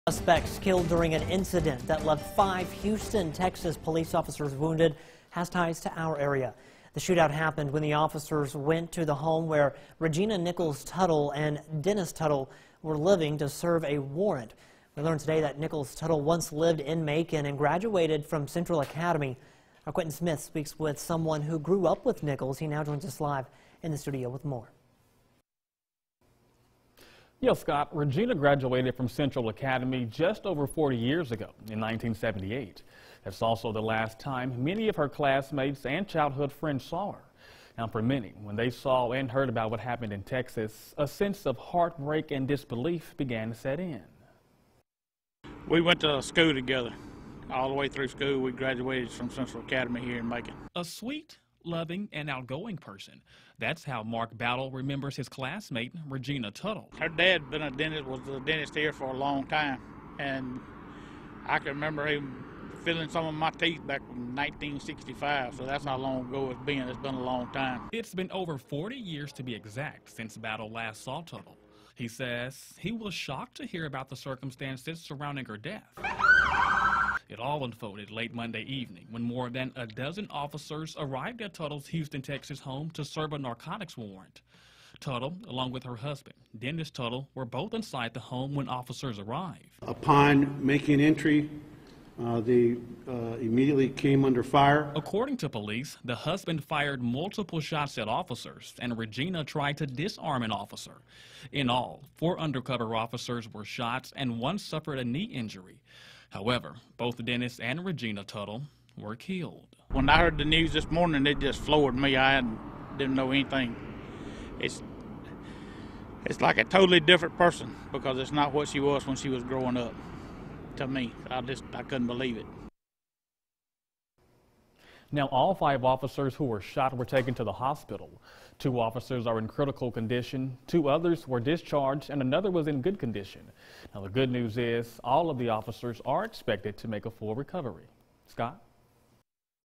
Houston Suspect
One of the suspects killed during an incident that left five Houston, Texas police officers wounded has ties to our area.